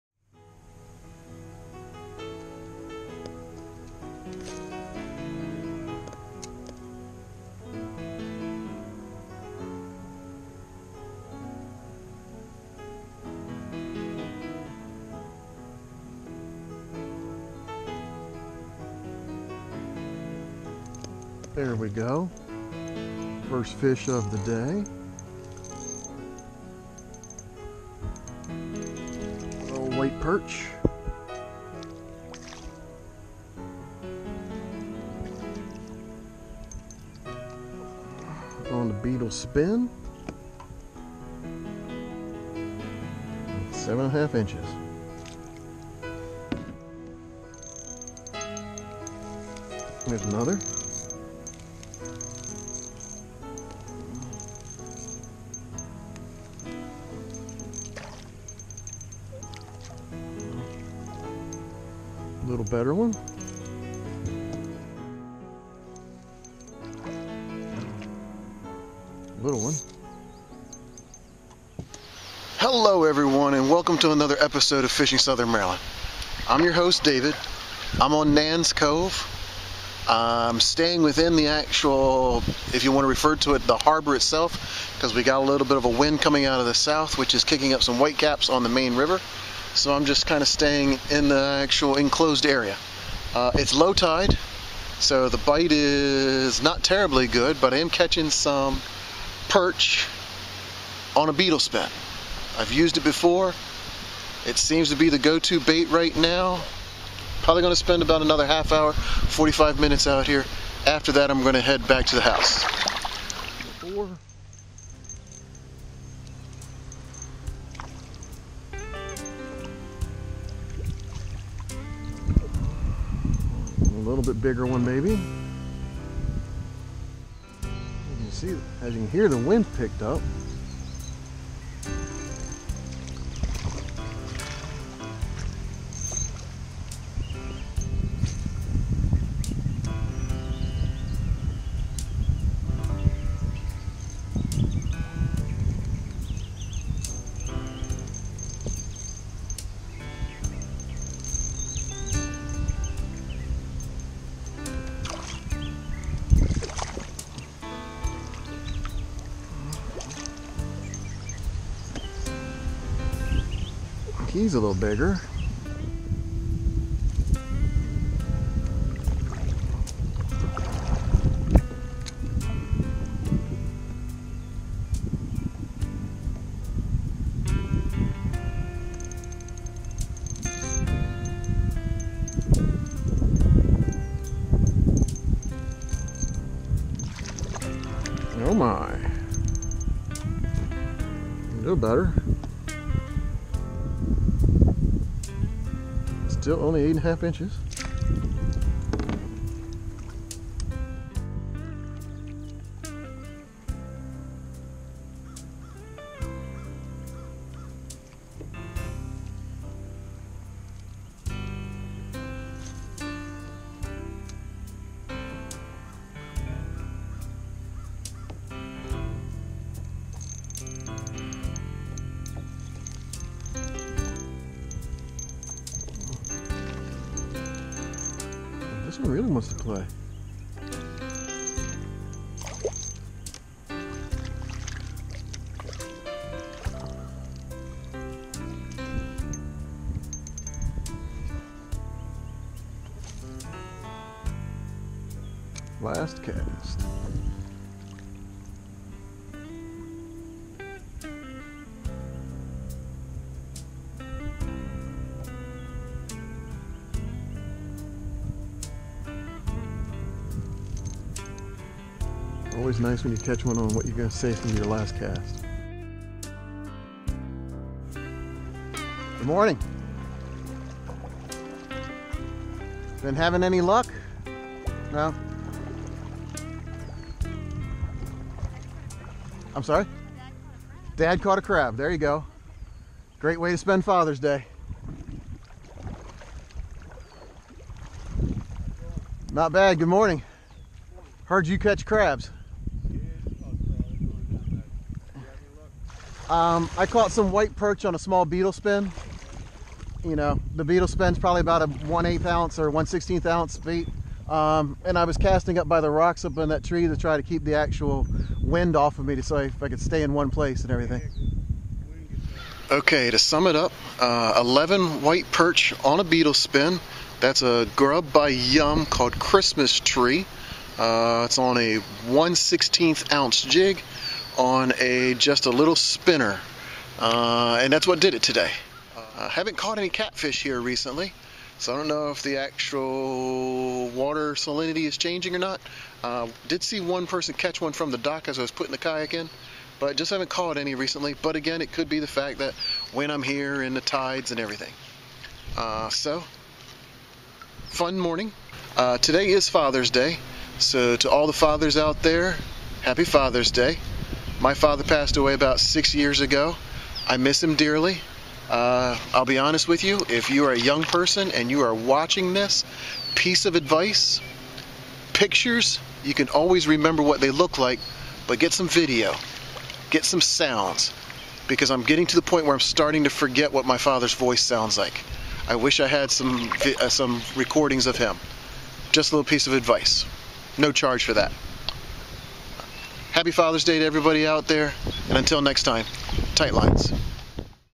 Fishing Southern Maryland: Happy Father's Day from Nan's Cove (June 16, 2019)
Happy Father's Day everyone!  I wanted to get this video out before the end of the day.  I spent a little time on Nan's Cove this morning and caught a few white perch.  Enjoy!